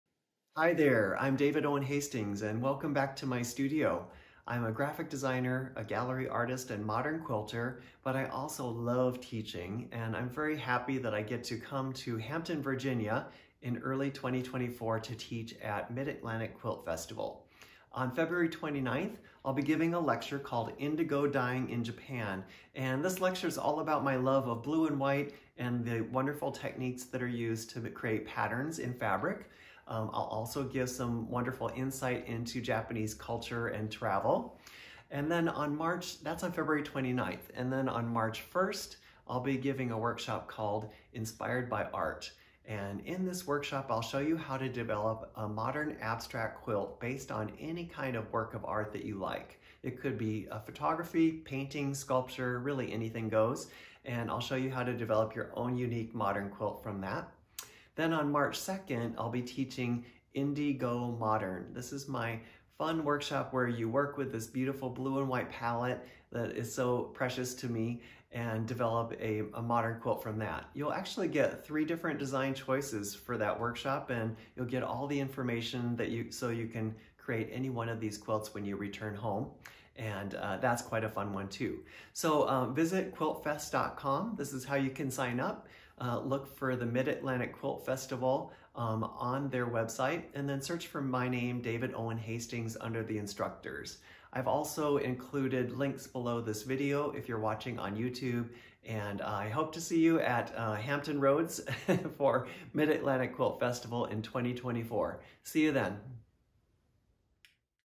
David Owen Hastings at the Mid-Atlantic Quilt Festival 2024
Hello modern quilters! I hope you’ll consider joining me at the Mid-Atlantic Quilt Festival, Feb. 29 – Mar. 3, 2024!

I am giving a lecture and two workshops, all in-person at the Hampton Roads Convention Center, Hampton, VA.

LECTURE: Indigo Dyeing in Japan  |  Feb. 29, 2024
Experience the Japanese art of dyeing fabric with natural indigo, and explore Japanese culture as well! This is a fun and informative lecture where you’ll learn about shibori (bound resist) and katazome (paste resist) dyeing techniques, plus lots of extra cultural information.

INSPIRED BY ART  |  Mar. 1, 2024
Starting with a favorite work of art, develop your own unique modern abstract quilt design. You’ll learn great improv techniques, and also get innovative ideas for how to quilt a modern design.

INDI-GO-MODERN  |  Mar. 2, 2024
Choose one of three modern designs, working with the classic blue and white palette. This is a great design class to help you notice things like value, pattern, scale and negative space in your modern quilts. We'll start with a simple stitched paper exercise to warm up!

I hope you'll join me for the lecture and two popular workshops. You’ll be inspired and enriched with new approaches to making modern quilts!

Thank you to Mancuso Show Management for hosting the Mid-Atlantic Quilt Festival in Hampton, VA. I look forward to seeing you in person!